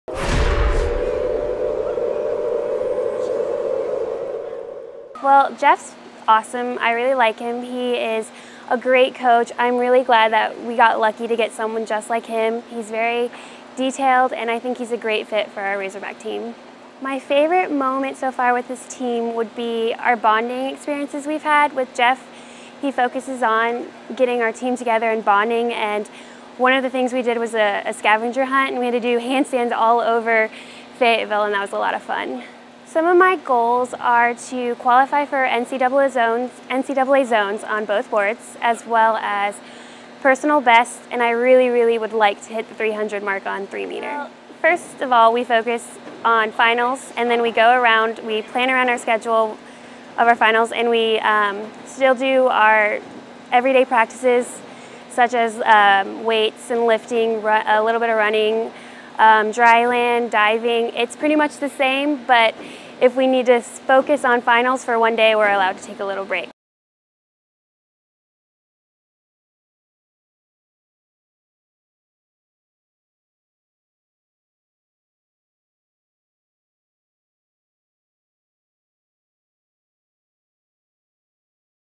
Clark Talks About the Diving Team
Meghan Clark talks about the diving team.